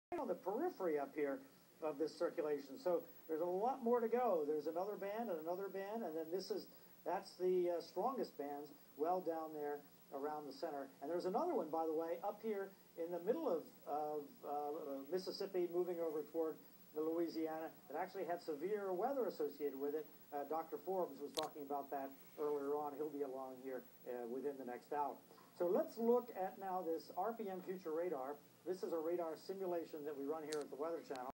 Hurricane Isaac strengthens as it takes aim at gulf coast.
This is an enormous hurricane, do not let category 1 or category 2 status fool you. The cat is not the thing to worry about, the surge along with up to 20 inches of rain over an extended period up to 36 hours will create mass destruction. Please evacuate now. Tornado warnings are in effect as well